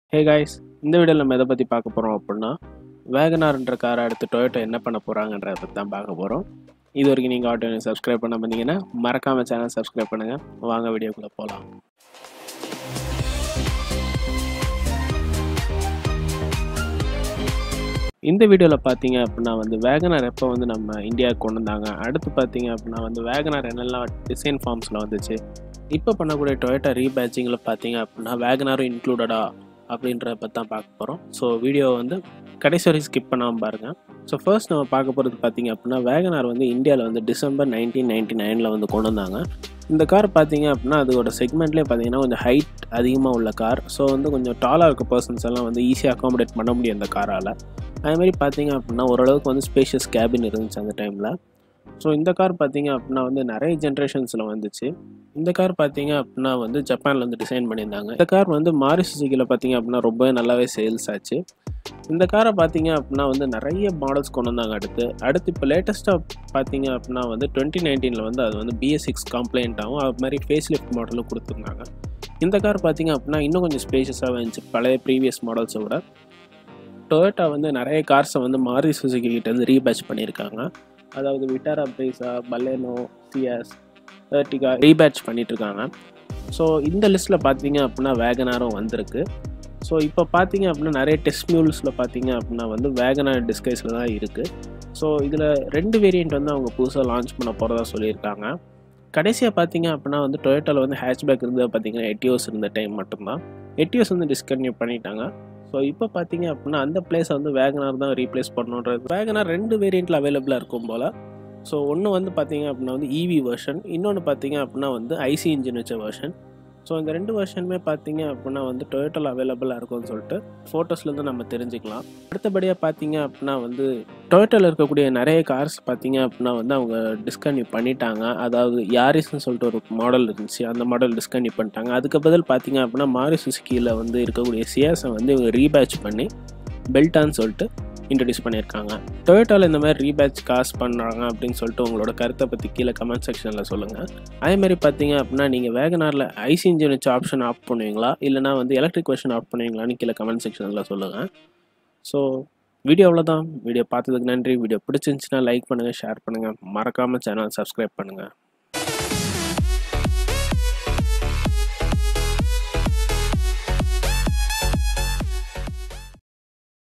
Toyota HyRyder....? | Tamil | Auto Venue | India
Hi internet
  Hope you guys are safe...! In this video we have covered the most exclusive maruti Suzuki Wagon R which has become a re- badged Toyota after the Belta AKA Ciaz. Make sure to watch the video without skipping  to know the updates regarding it.

Cheers,
Auto Venue Team